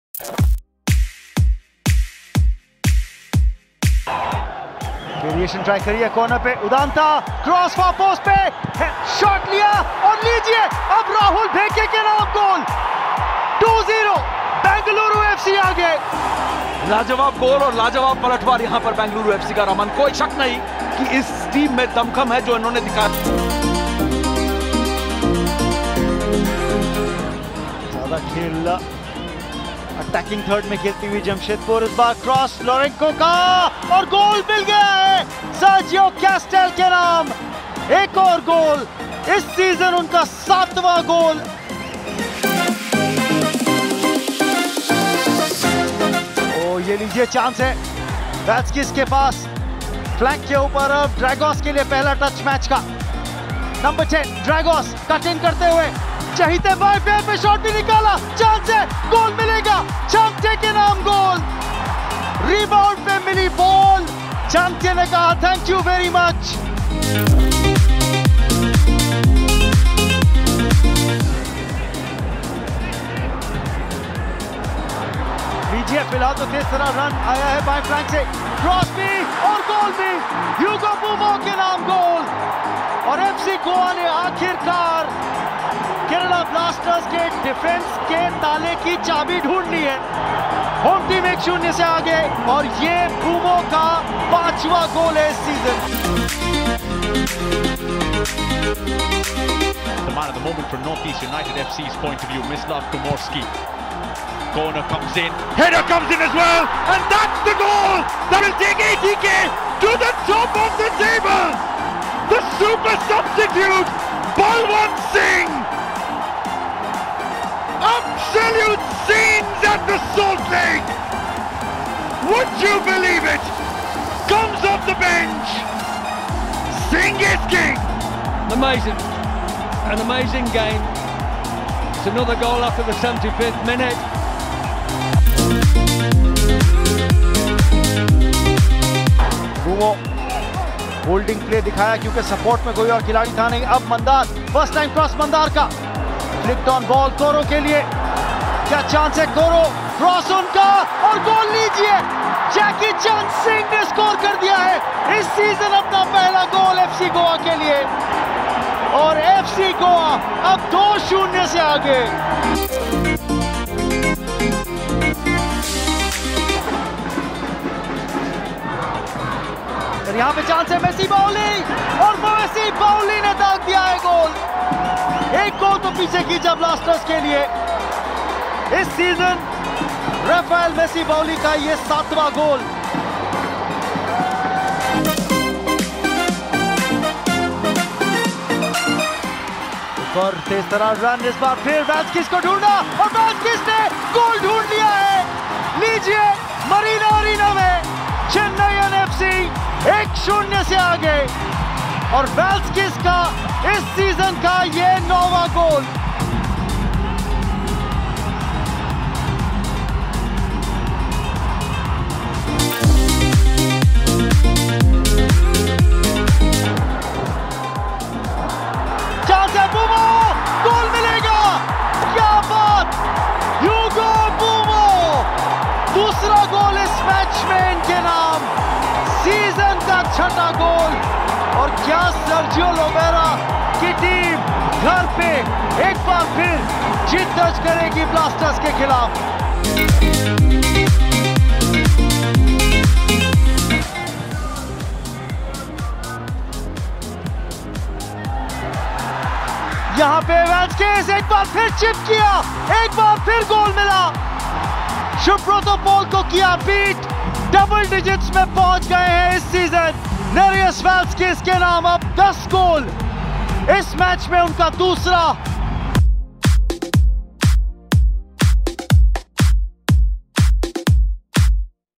ISL 2019-20 Week 12 Top 10 Goals ft. Messi, Balwant Singh & Nerijus Valskis
With 16 goals coming, the 12th week in Indian Super League turned out to be a highly memorable one. The last gasp-winner against NorthEast United from Balwant Singh helped ATK grab the top spot again as the extraordinary title race continues. Below them stand FC Goa, only inferior by goal difference. The Gaurs too had a huge result against Kerala Blasters with a 3-2 win at home. On the other hand, Bengaluru FC showed Odisha FC their class with a 3-0 win at home.

Another huge result in the week came in Chennai as Chennaiyin FC thrashed Jamshedpur 4-1 to dent the Men of Steel's hopes of top four finish. Finally at the Gachibowli stadium, Hyderabad FC delivered a huge upset to Mumbai City FC with a 1-1 draw after going down in the first half. Check out the top 10 strikes from the week!

Video credit- ISL